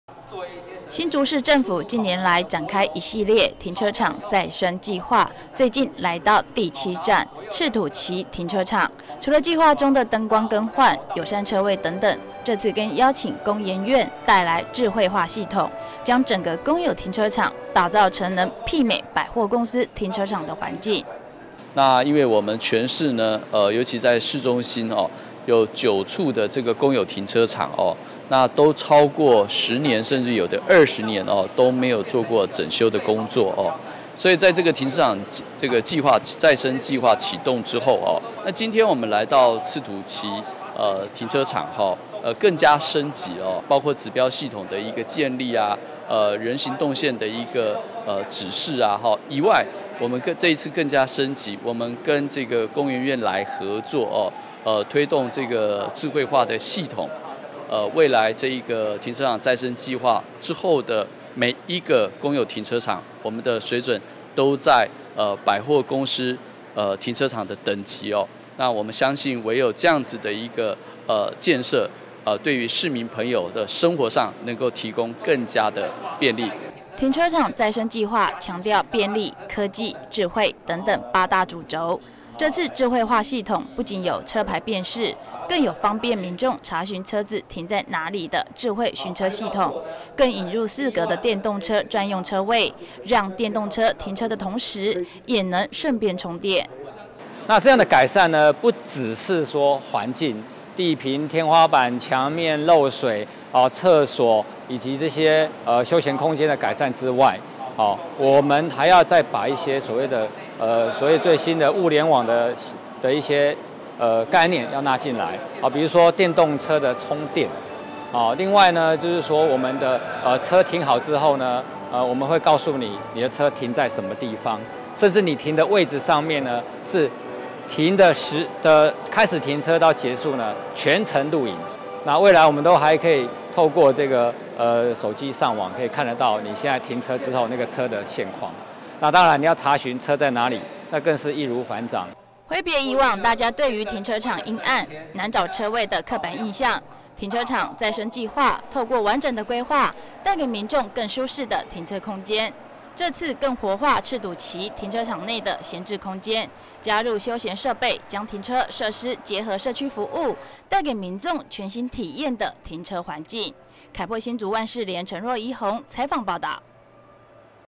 20180326N 停車場再生計畫第七站 赤土崎嶄新亮相
新竹市目前有九個公有停車場，啟用時間都超過十年甚至二十年，近年來新竹市政府大力推動停車場再生計畫，不僅增加燈光，重新規劃動線，最近第七站赤土崎停車場正式啟用，更引入工研院資源，帶來新竹市第一座有電動車充電站的停車場，更帶來最新的設備，從車牌辨識、智慧尋車，期望將公有停車場打造成能媲美百貨公司停車場的環境。